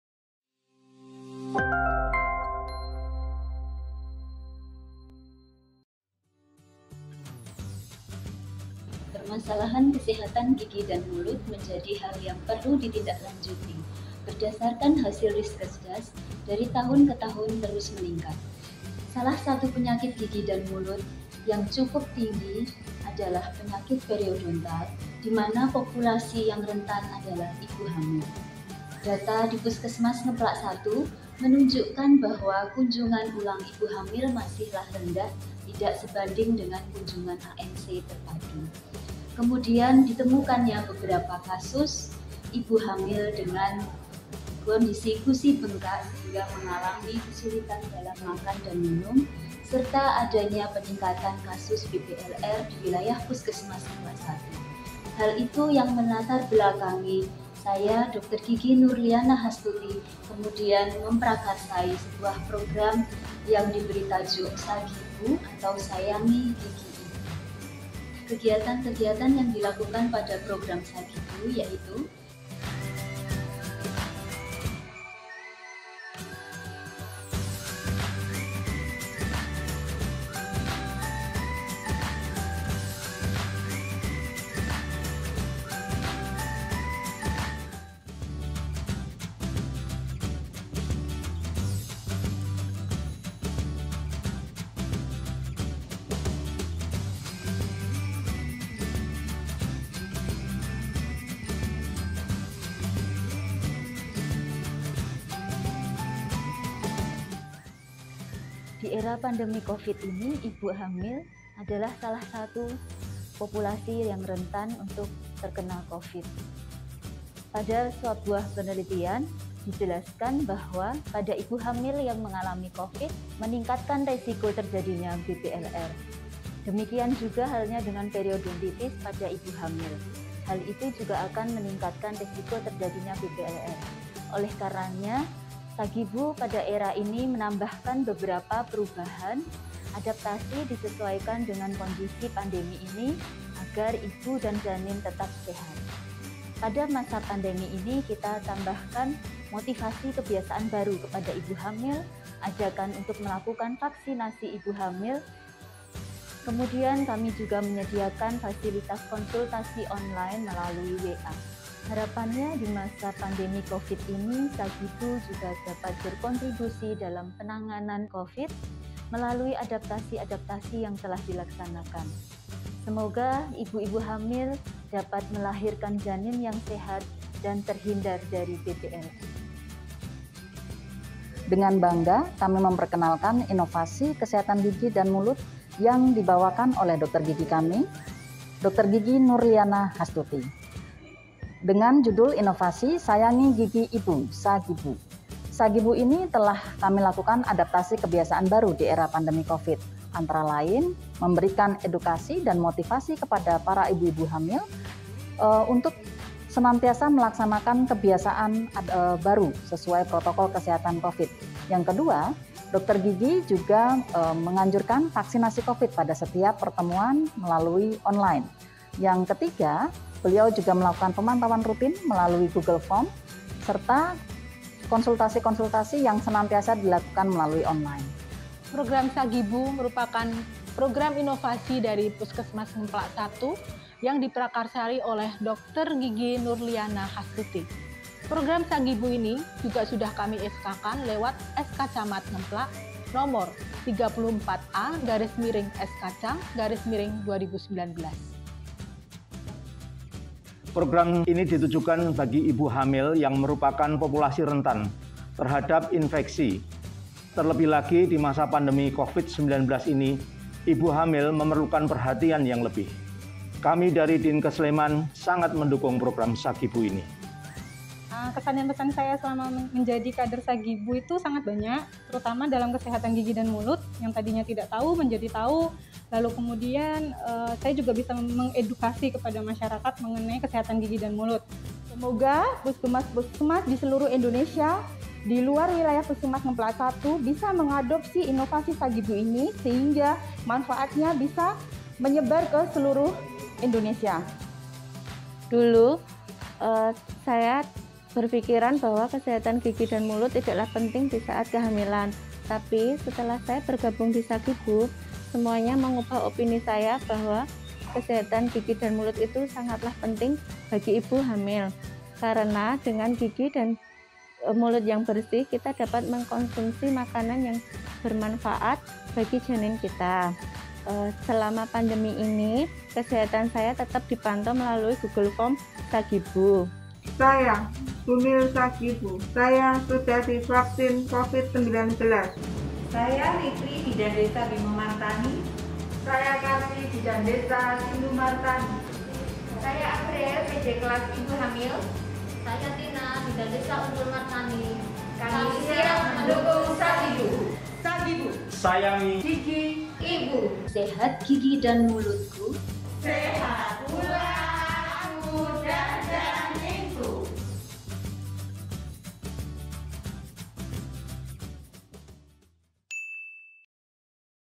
SAGIBU ( Sayangi Gigi Ibu )
SAGIBU (Sayangi Gigi Ibu) sebagai salah satu program inovasi di Puskesmas Ngemplak 1, membuat beberapa langkah terobosan dalam upaya percepatan penanganan Covid19 di wilayah Puskesmas Ngemplak 1.

Diharapkan melalui upaya yang dilakukan dapat meningkatkan derajat kesehatan ibu hamil, meningkatkan capaian vaksinasi covid19 bagi bumil, dan juga menurunkan risiko BBLR di wilayah Puskesmas Ngemplak 1

SAGIBU (Sayangi Gigi Ibu) untukmu Bumilku (ibu hamilku), bersama cegah Covid19 dan BBLR